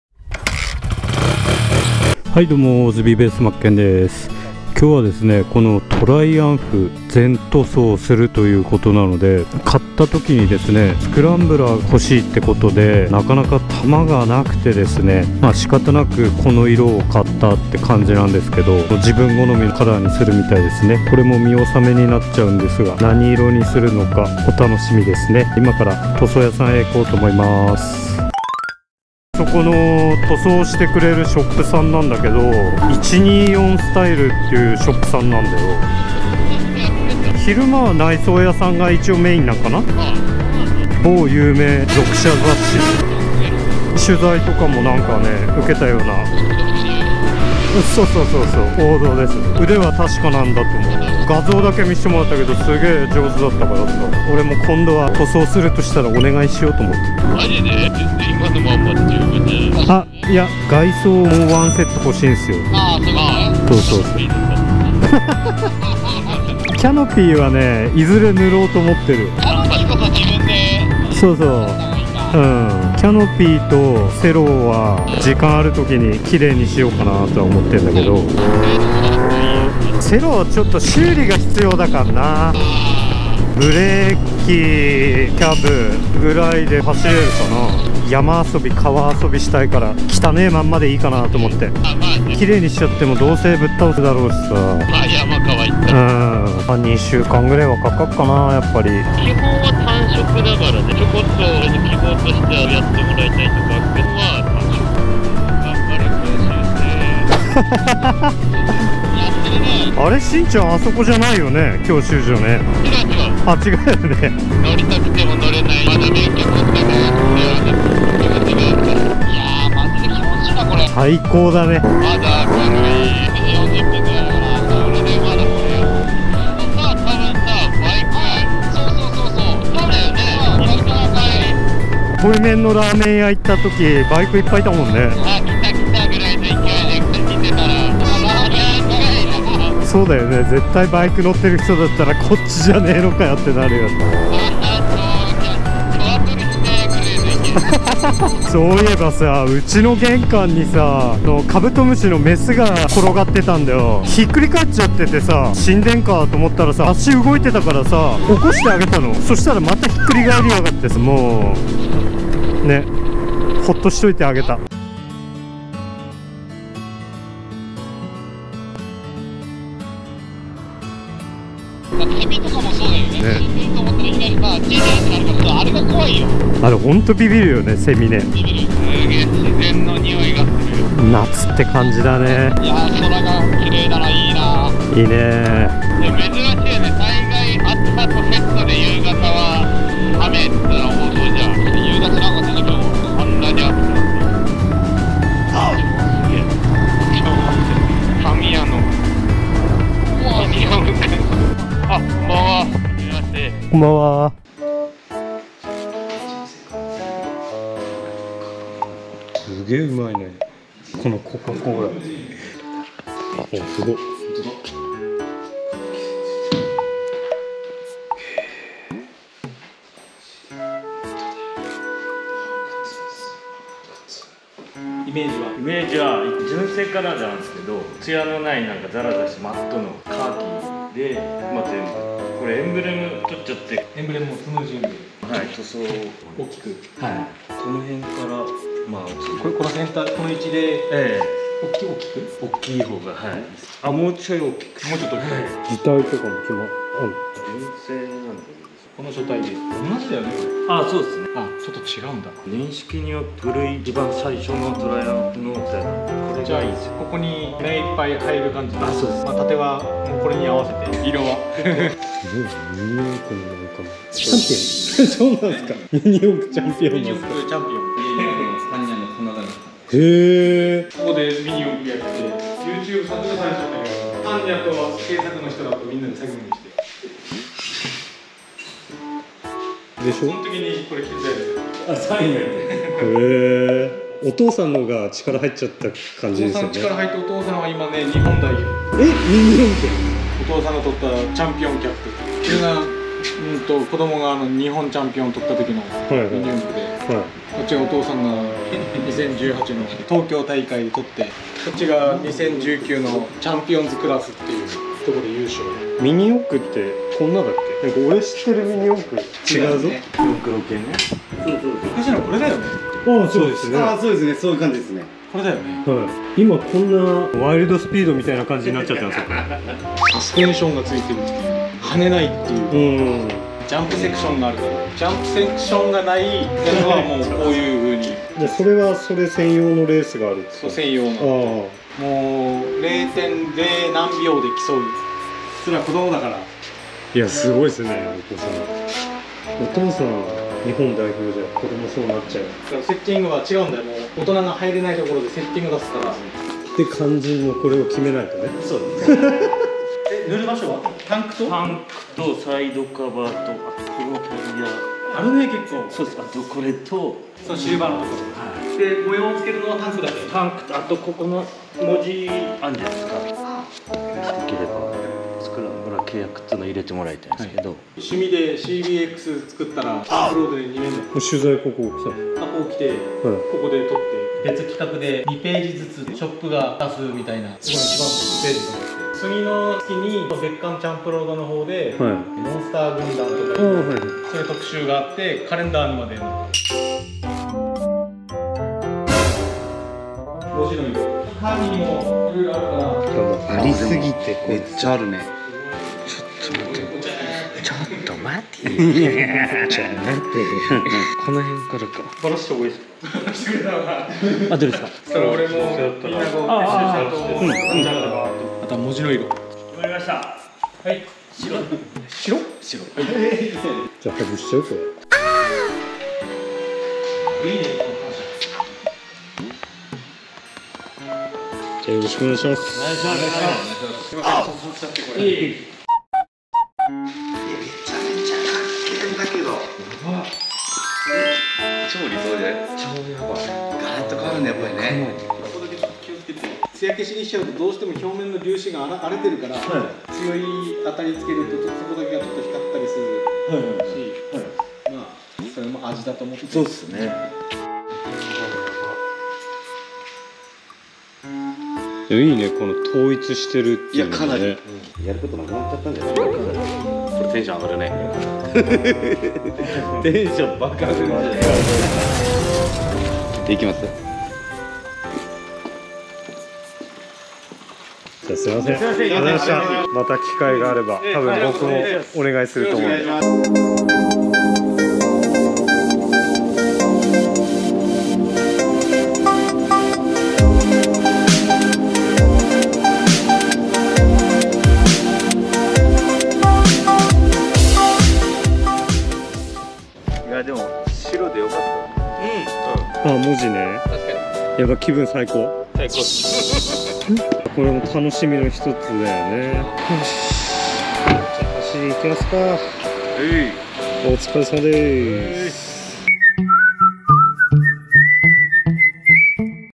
カスタムペイントショップのオーナーさんはミニ四駆も凄かった
友人のトライアンフスクランブラーをオールペンする為にカスタムペイントショップ124styleさんへ行って来ました。

塗装の依頼はこちら
カスタムペイント　124style
栃木県那須烏山市福岡582-13